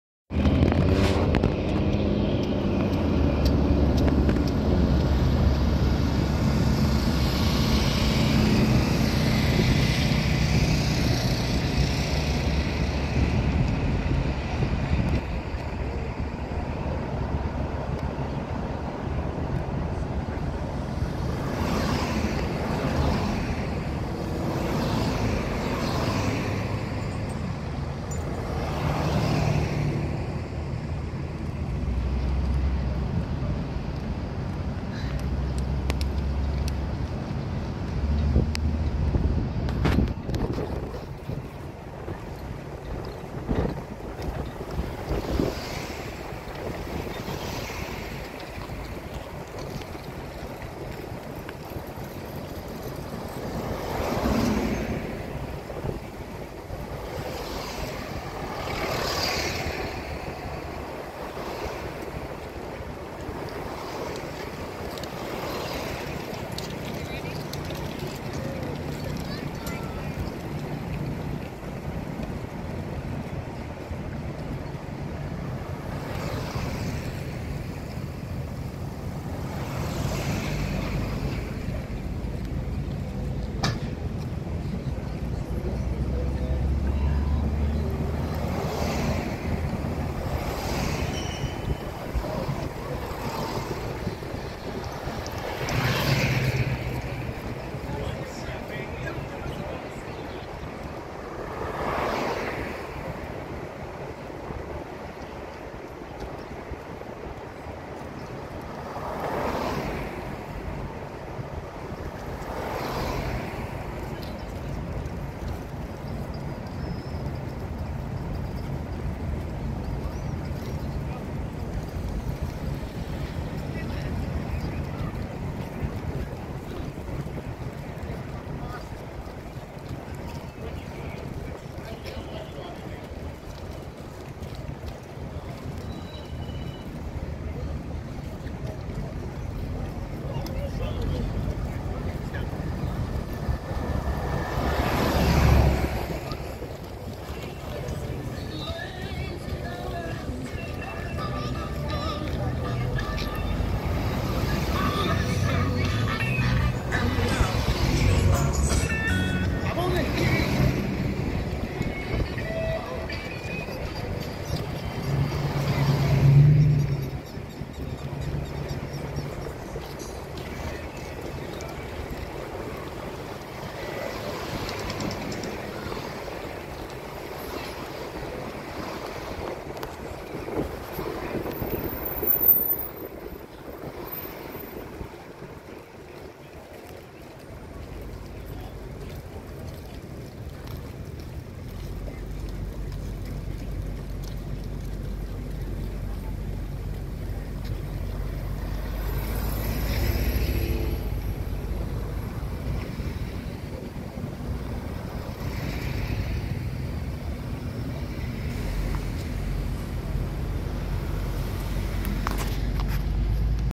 packwood 2023
Packwood Flea Market, Friday, May 26th. A drive down one side.